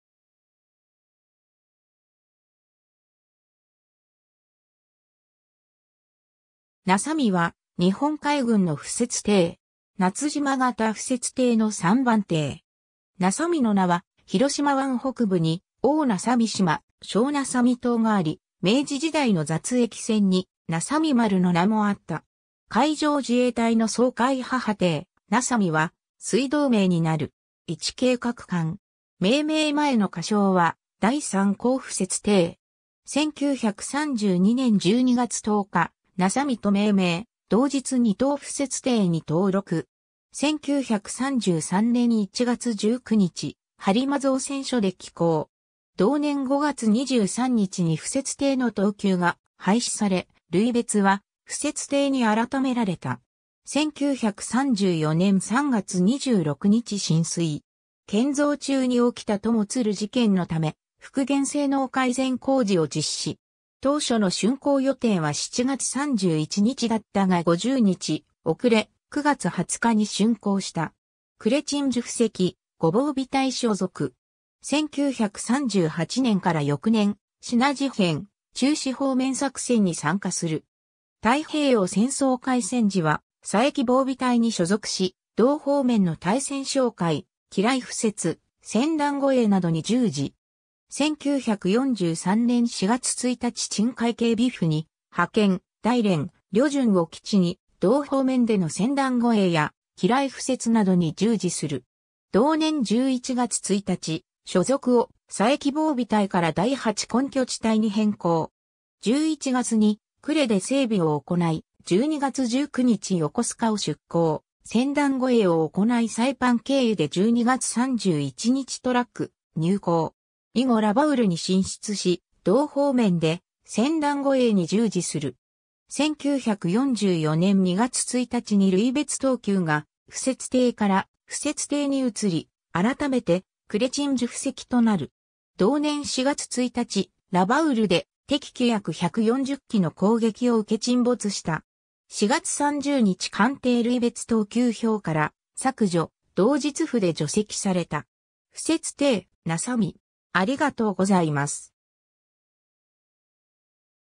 那沙美 (敷設艇)
那沙美(なさみ)は、日本海軍の敷設艇。
夏島型敷設艇の3番艇。
 那沙美の名は広島湾北部に大那沙美島・小那沙美島があり、明治時代の雑役船に「那沙美丸」の名もあった。
海上自衛隊の掃海母艇「なさみ」は水道名になる。
 ①計画艦。
命名前の仮称は「第三號敷設艇」。
1932年(昭和7年)12月10日「那沙美」と命名、同日二等敷設艇に登録。
1933年(昭和8年)1月19日播磨造船所で起工。
同年5月23日に敷設艇の等級が廃止され、類別は(等級無しの)敷設艇に改められた。
1934年(昭和9年)3月26日進水。
建造中に起きた友鶴事件のため、復元性能改善工事を実施。
当初の竣工予定は7月31日だったが50日遅れ、9月20日に竣工した。
呉鎮守府籍、呉防備隊所属(1936年時)。
 1938年(昭和13年)から翌年、支那事変、中支方面作戦に参加する 。
 太平洋戦争開戦時は佐伯防備隊に所属し同方面の対潜哨戒、機雷敷設、船団護衛などに従事。
 1943年(昭和18年)4月1日鎮海警備府に派遣、大連・旅順を基地に同方面での船団護衛や機雷敷設などに従事する。
同年11月1日、所属を佐伯防備隊から第八根拠地隊に変更。
11月に呉で整備を行い、12月19日横須賀を出港、船団護衛を行いサイパン経由で12月31日トラック入港。
以後ラバウルに進出し同方面で船団護衛に従事する。
 1944年(昭和19年)2月1日に類別等級が(特務艇の)敷設艇から(艦艇の)敷設艇に移り、改めて呉鎮守府籍となる。
 同年4月1日、ラバウルで敵機約140機の攻撃を受け沈没した。
4月30日艦艇類別等級表から削除、同日附で除籍された。
 敷設艇「那沙美」。